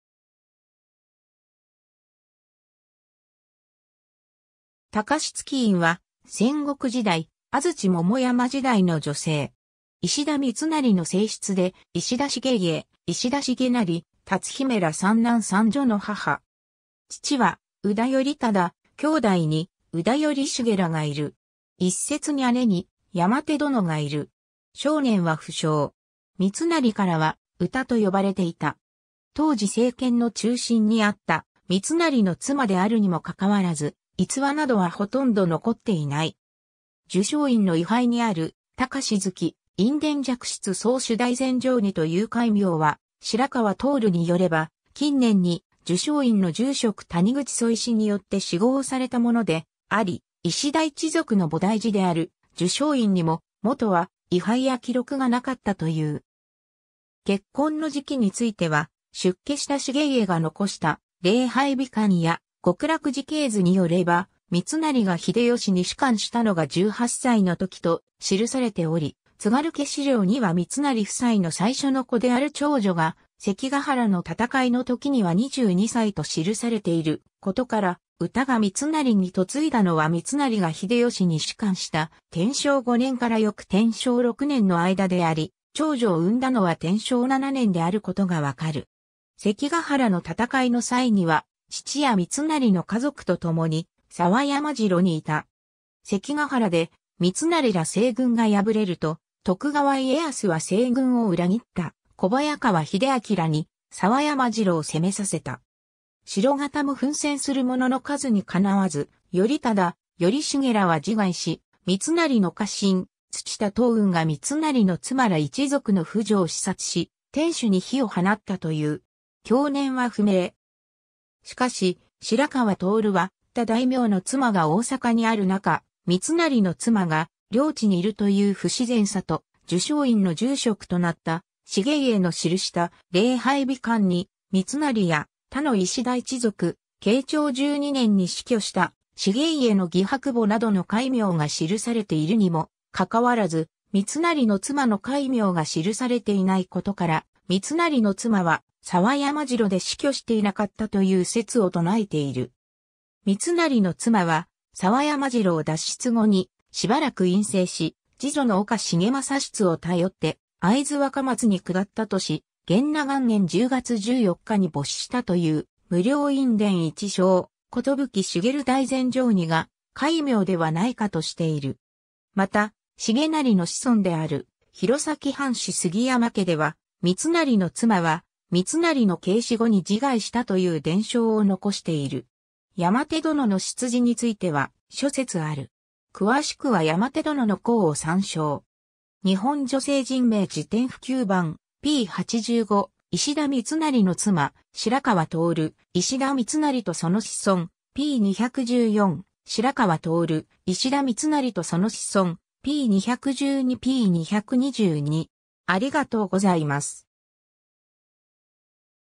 皎月院
皎月院（こうげついん、? - 慶長5年9月17日（1600年10月23日））は、戦国時代・安土桃山時代の女性。
石田三成の正室で石田重家、石田重成、辰姫ら3男3女の母（後妻であり二男一女の母とも）。
父は宇多頼忠、兄弟に宇多頼重らがいる。
一説に姉に山手殿（真田昌幸正室） がいる。
 生年は不詳。
三成からは「うた」と呼ばれていた。
当時政権の中心にあった三成の妻であるにもかかわらず、逸話などはほとんど残っていない。
 寿聖院の位牌にある「皎月院殿寂室宗珠大禅定尼」という戒名は、白川亨によれば、近年に寿聖院の住職谷口楚石によって諡号されたものであり、石田一族の菩提寺である寿聖院にも、もとは位牌や記録がなかったという。
 結婚の時期については、出家した重家が遺した『霊牌日鑑』や『極楽寺系図』によれば、三成が秀吉に仕官したのが18歳の時と記されており、『津軽家史料』には三成夫妻の最初の子である長女（山田隼人正の妻）が関ヶ原の戦いの時には22歳と記されていることから、うたが三成に嫁いだのは三成が秀吉に仕官した天正5年（1577年）から翌天正6年（1578年）の間であり、長女を産んだのは天正7年（1579年）であることがわかる。
 関ヶ原の戦いの際には父や三成の家族とともに佐和山城にいた。
関ヶ原で三成ら西軍が敗れると、徳川家康は西軍を裏切った小早川秀秋らに佐和山城を攻めさせた。
城方も奮戦するものの数に敵わず、頼忠、頼重らは自害し、三成の家臣・土田桃雲が三成の妻ら一族の婦女を刺殺し、天守に火を放ったという。
享年は不明。
 しかし、白川亨は他大名の妻が大坂にあるなか三成の妻が領地にいるという不自然さと、寿聖院の住職となった重家（宗亨）の記した『霊牌日鑑』に三成や他の石田一族、慶長12年（1607年）に死去した重家の義伯母（石田正澄の妻）などの戒名が記されているにもかかわらず、三成の妻（重家の母）の戒名が記されていないことから、三成の妻は佐和山城で死去していなかったという説を唱えている。
三成の妻は佐和山城を脱出後にしばらく隠棲し、次女の岡重政室を頼って会津若松に下ったとし、元和元年（1615年）10月14日に没したという「無量院殿一相寿卯大禅定尼」が戒名ではないかとしている。
 また、重成の子孫である弘前藩士杉山家では、三成の妻は三成の刑死後に自害したという伝承を残している。
 ^ 山手殿の出自については諸説ある。
詳しくは山手殿の項を参照。
 ^ 『日本女性人名辞典　普及版』（日本図書センター、1998年）p85「石田三成の妻」 ^ 白川亨『石田三成とその子孫』p214 ^ 白川亨『石田三成とその子孫』p212-ｐ222